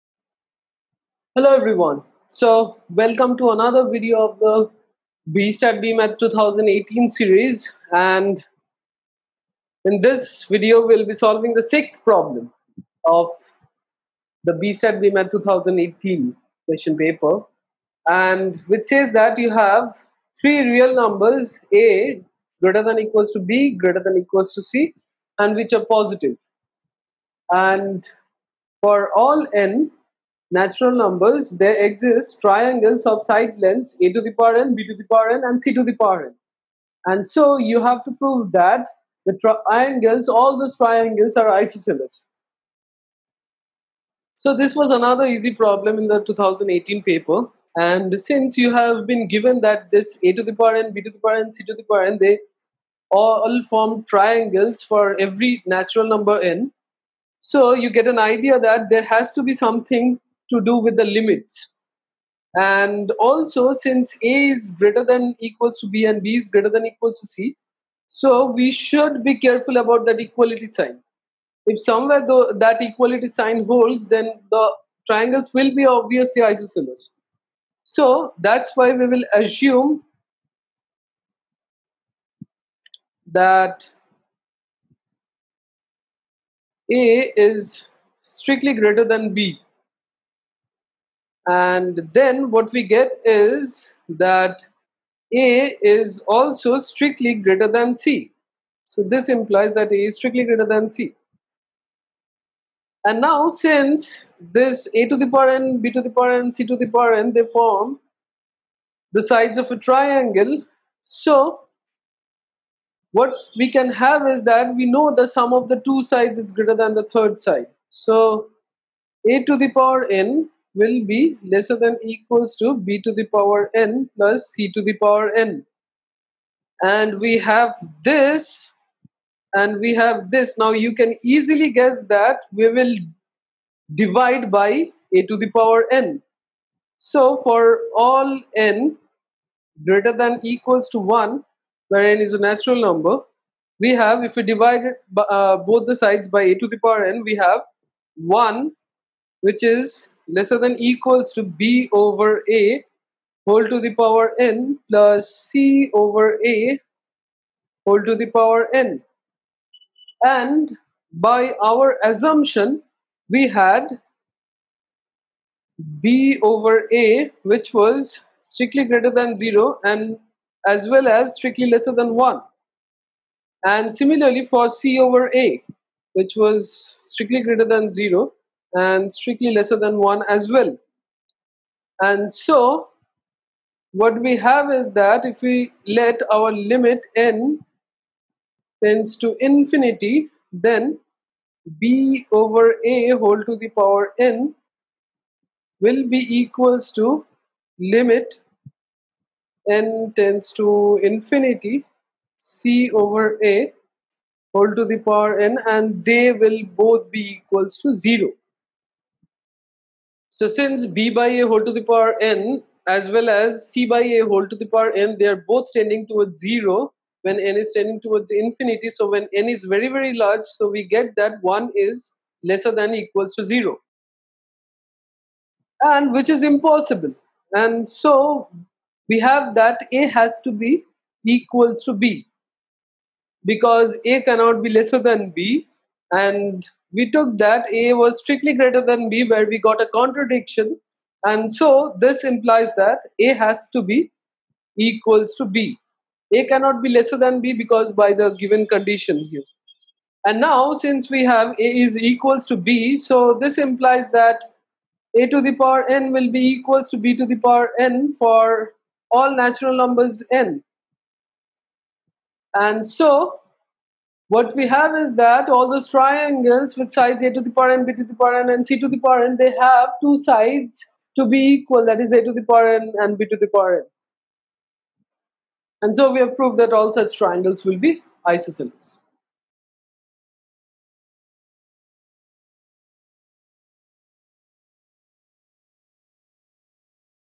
Indian Statistical Institute, ISI BStat/BMath 2018 Solution & Discussion: Problem 6
For the full set of problems and hints and solutions
Indian Statistical Institute (I.S.I.) Subjective UGB B.Stat. & B.Math. 2018 Problem 6 Solution & Discussion
All B.Stat & B.Math 2018 UGB Hints along with solution and discussion